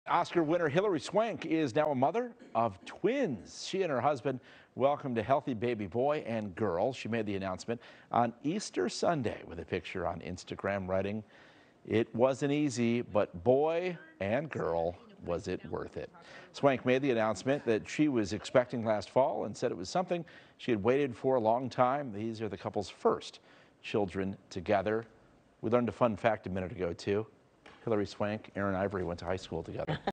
Hilary Swank gives birth to twins, shares first photo
Hilary Swank has given birth to twins — a boy and a girl.